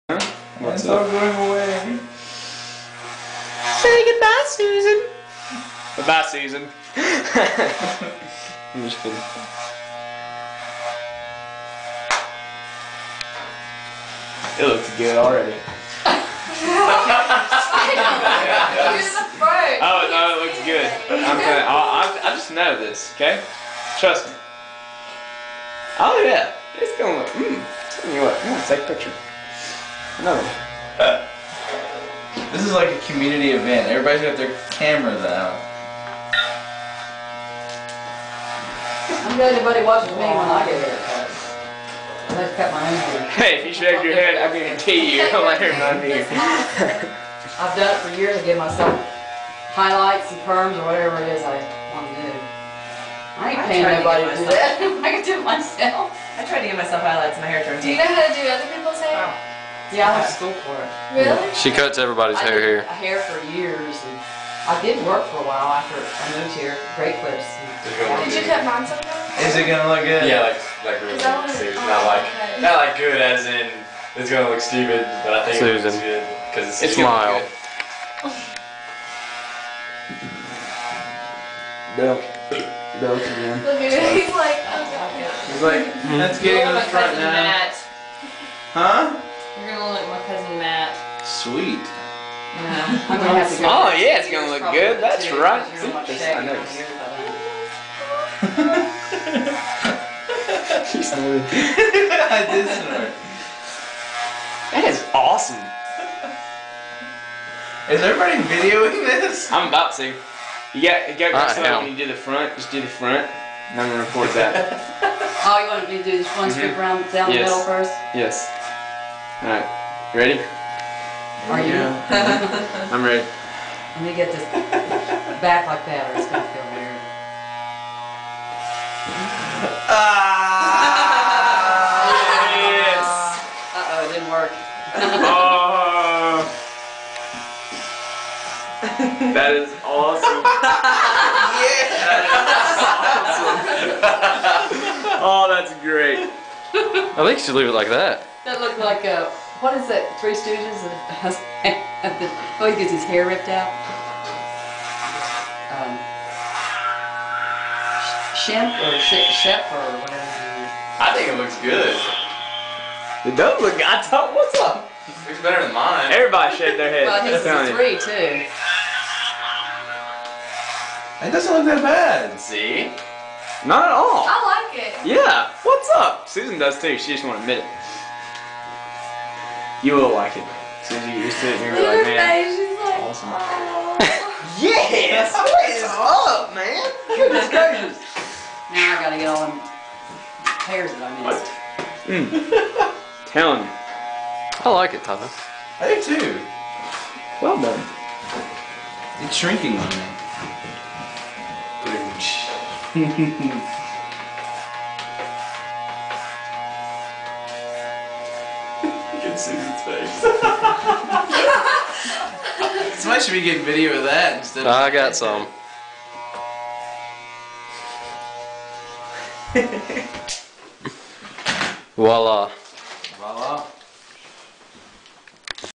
Tyler's Buzz
Buddy Tyler is talked into getting a buzz